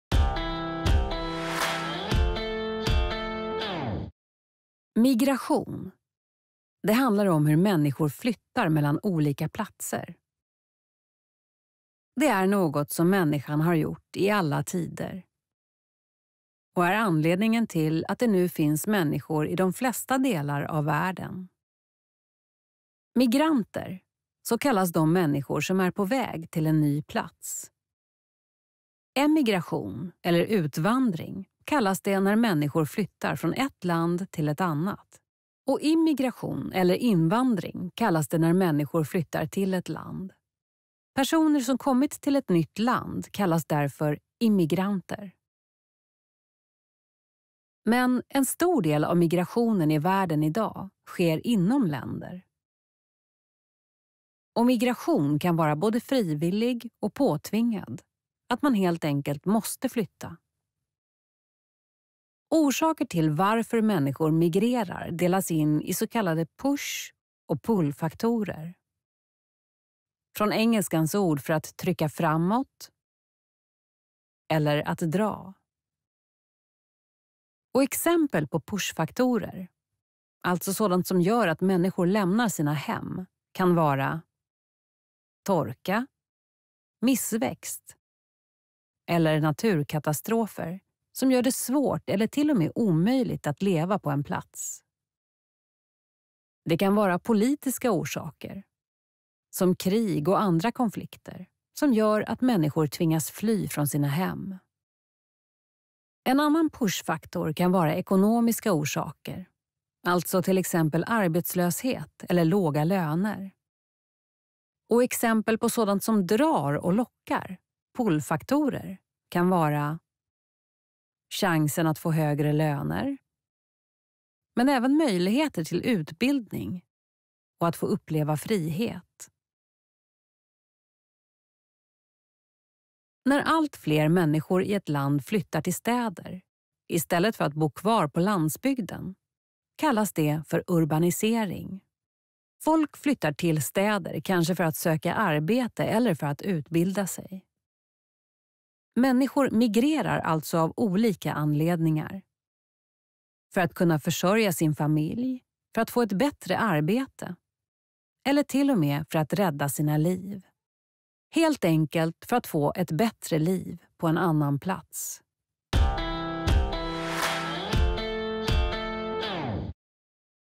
Vad är migration?
I denna Begreppafilm svarar vi på frågan "Vad är migration?"